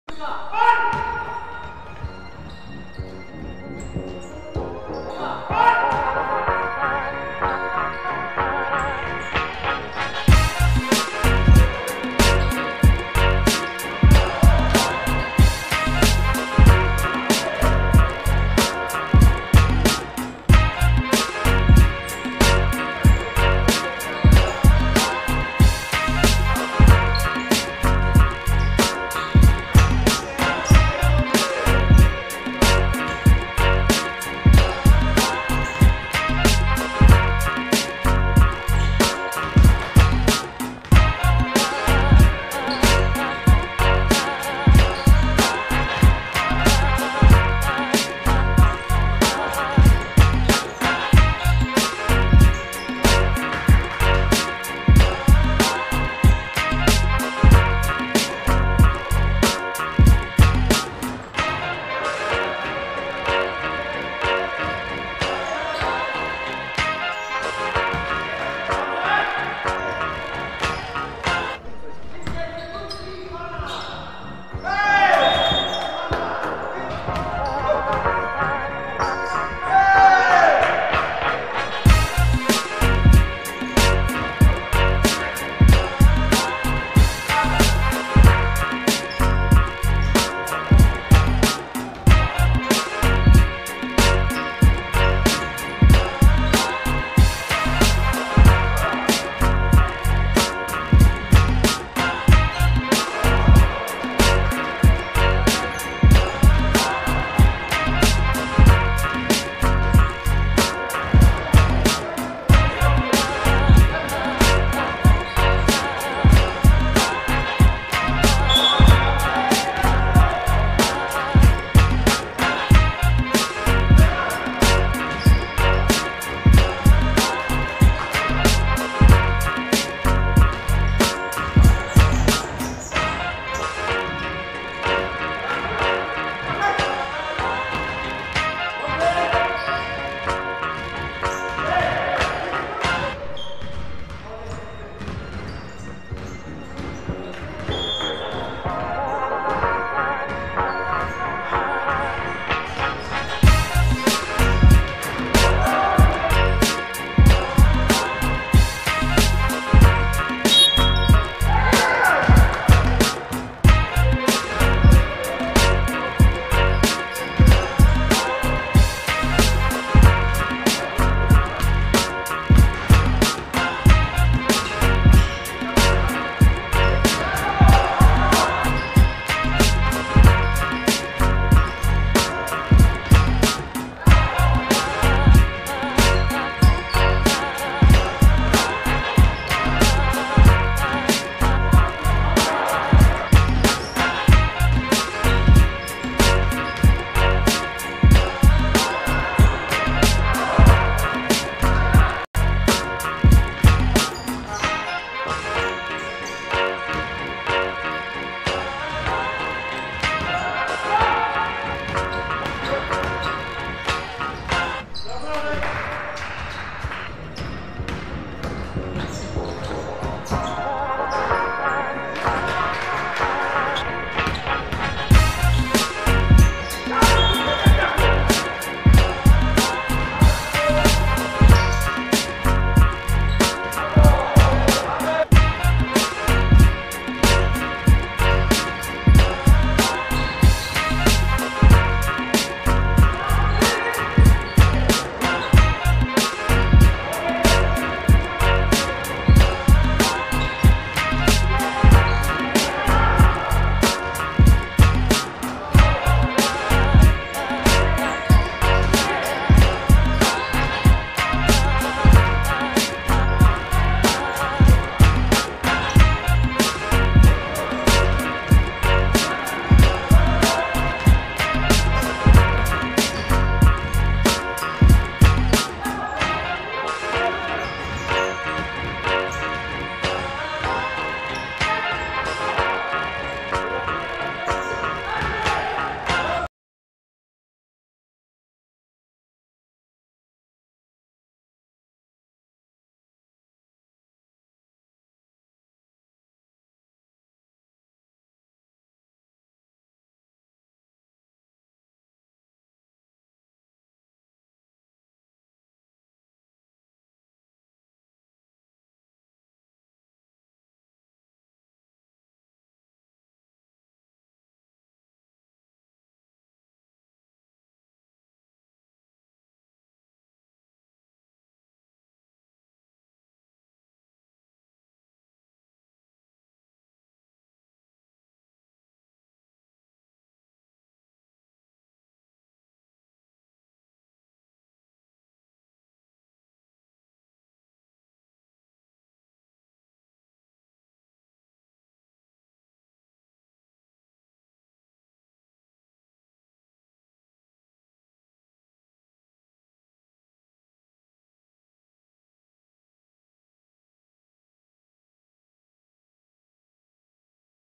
13 12 Njersey vs San Antonio – Basket 36+ Ursynów
NJERSEY 67:64 SAN ANTONIO
M.Kosierb 43pkt 8x3 21zb
P.Rabiński 12pkt 8zb 9ast